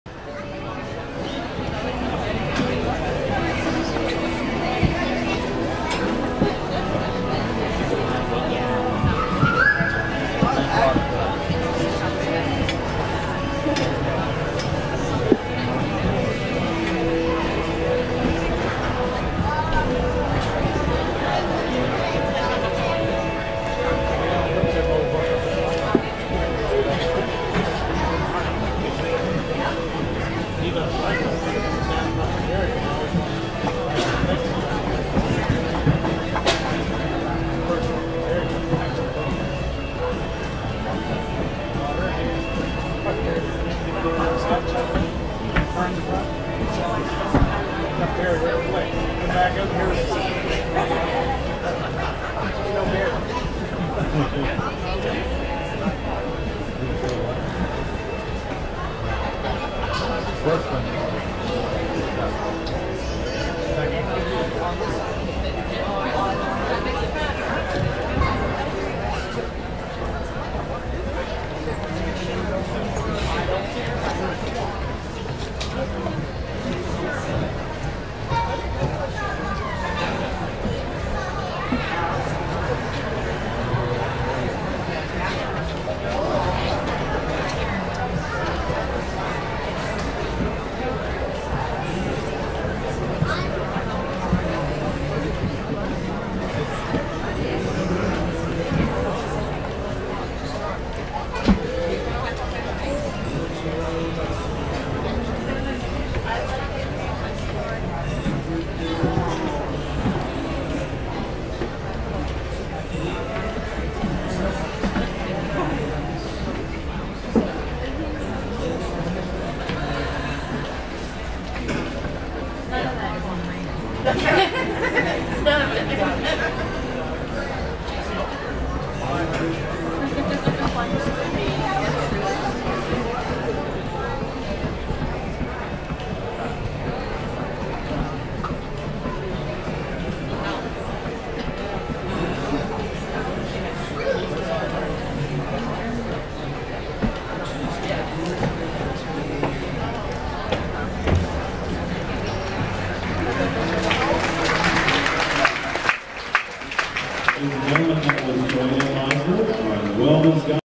Hants County Ex 2014 - Trail Ride Challenge 4 of 10
A display of skill in the Trail Ride Challenge at the Hants County Exhibition as horse and rider work their way through a series of challenging obstacles.

For more details on the Hants County Ex visit its